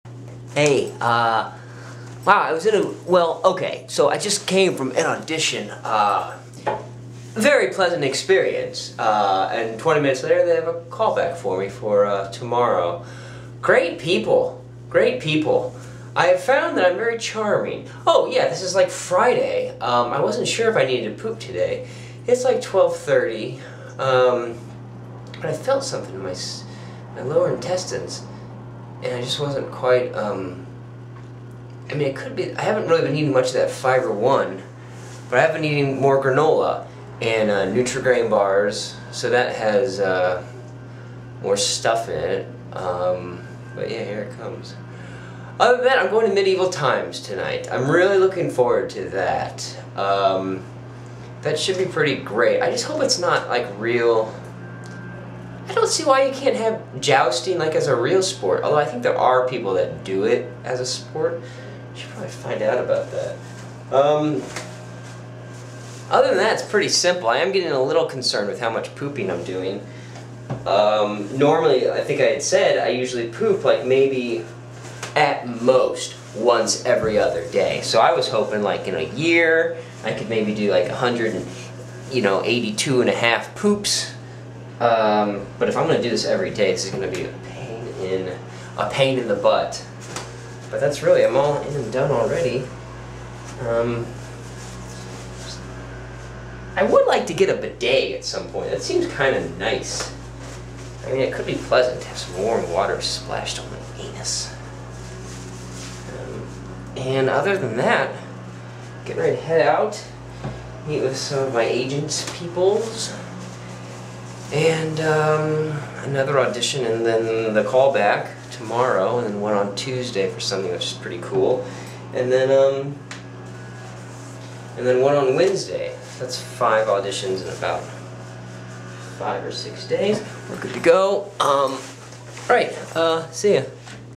Poop Confessionals Vol 3 - New Diets Suck
All this this pooping is putting a cramp in my style.